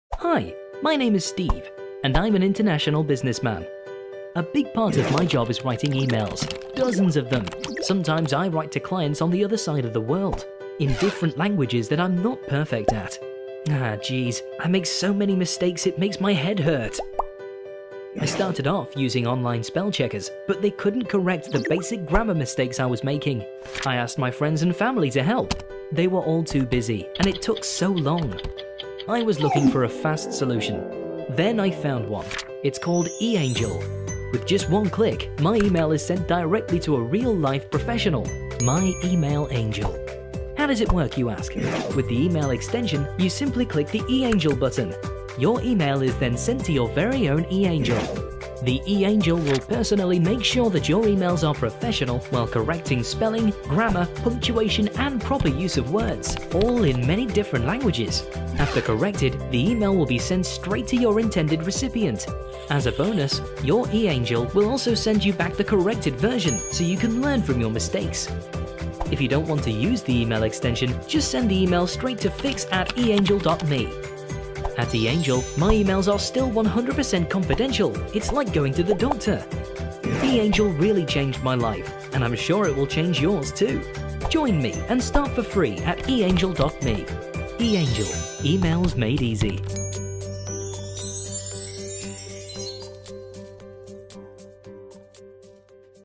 Proofread your Texts & Emails with eAngel
Our angels will correct and proofread your texts and emails in minutes.

Texts made easy.

We all know that a professional level of writing cannot simply be achieved with an automated spelling or grammar checker. No matter how advanced automated software has become it still cannot replace the human brain. At eAngel.me each text is corrected personally by one of our language experts.

Our vision is moving beyond language skills and creating clear channels of communication. We believe that different abilities, languages and countries should not be a barrier for accurate correspondence. We want to make it easier and faster, for business professionals, students and individuals living with disabilities interfering with language skills like dyslexia, to efficiently communicate. eAngel.me offers every person just that, and at a low, affordable cost.

With one simple click your text or email is sent to one of our angels, who personally corrects it and makes sure you sound professional. We correct (not translate) spelling, grammar, punctuation and proper use of words. We grammar correct texts in English, Spanish, French, German and Hebrew.

eAngel.me is suited to both business and private users. eAngel.me supports any type of text and any type of browser.

Your personal details, your recipients' details and any attachments are never seen by our angels. We only send the body text of your emails to our angels to help us ensure your privacy. We are committed to your confidentiality.

Our pricing model adapts to your needs. If your writing skills are at a high level and you only require minor corrections you will pay less than a user whose writing requires a high number of corrections.

There are a few ways you can use our proofreading services.

If it's a plain text, you can simply paste it or write it directly in the text box which is found on our

If it's a word file (.docx), you can upload it here , or use our Word add-in

If it's a presentation, you can upload it here , or use our PowerPoint add-in

If you are Google Docs, we have an add-on for it

If you are using Outlook, we have an add-in add-in for it

If you are using any email client, you can always simply send the email to , and we will correct it for you.

For more ways, look at the Services section on our website footer.

-The eAngel team